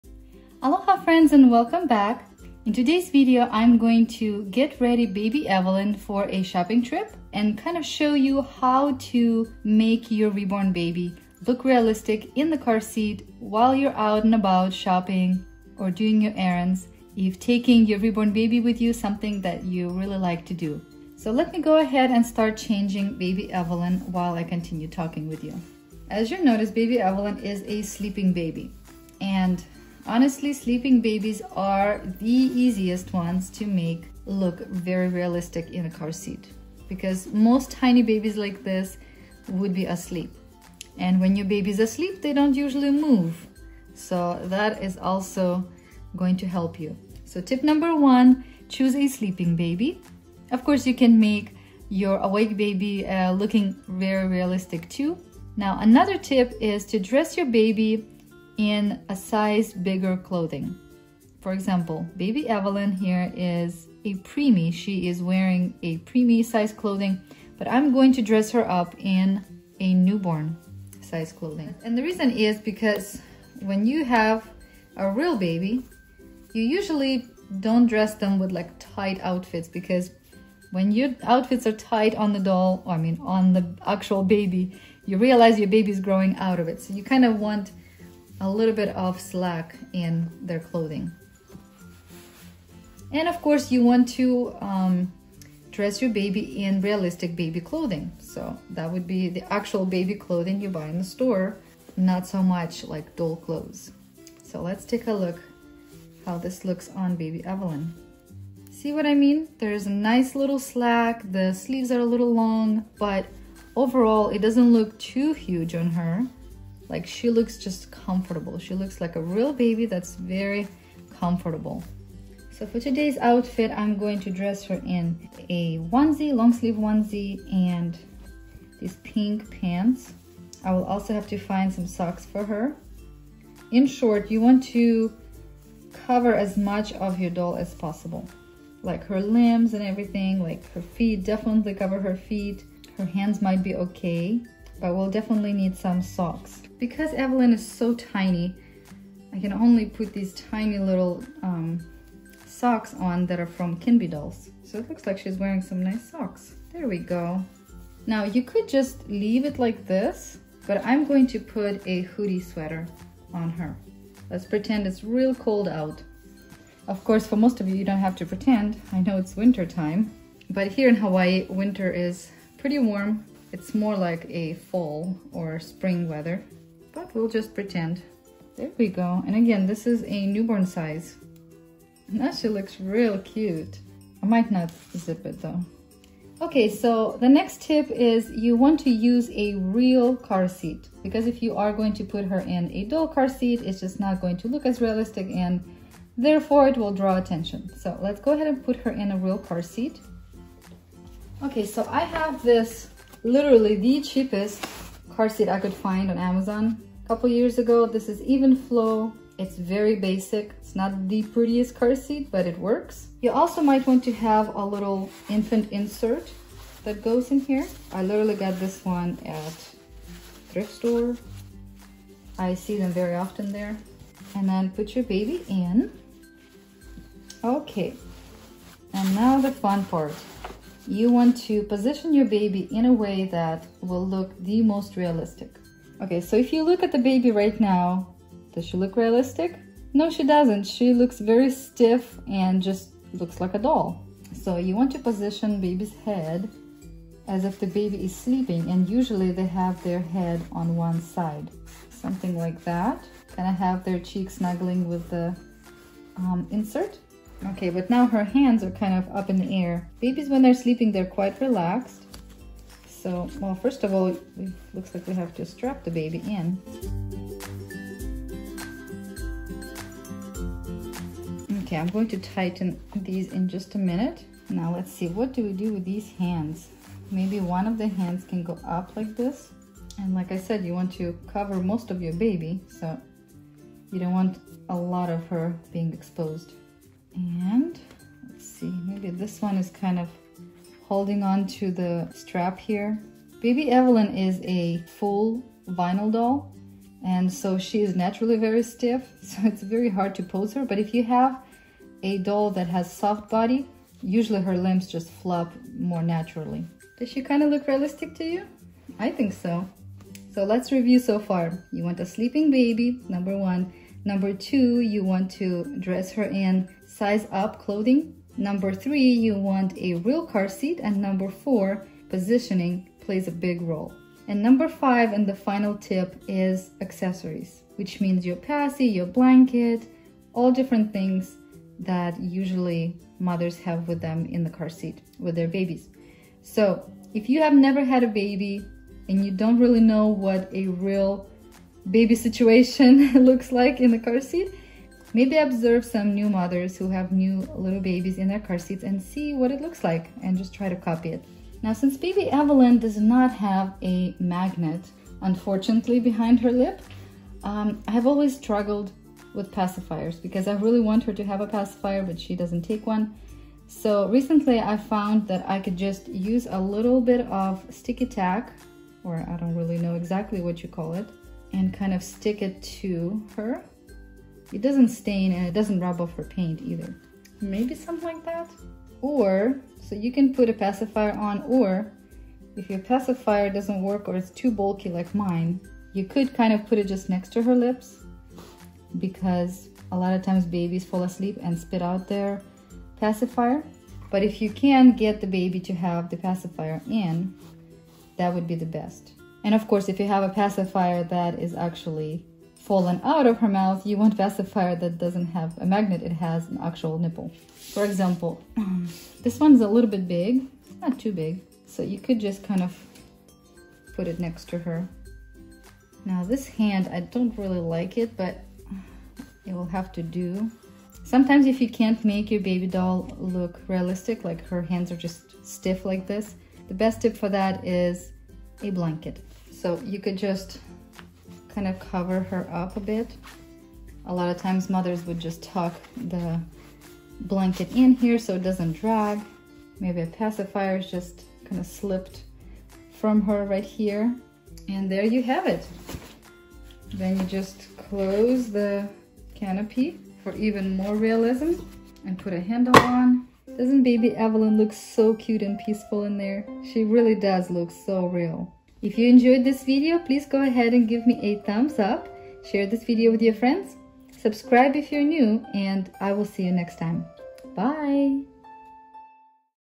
5 Tips To Make Your Sleeping Reborn Baby Look Realistic In The Carseat.
5 Tips To Make Your Sleeping Reborn Baby Look Realistic In The Carseat.

-Click The 🔔 To Never Miss A New Video!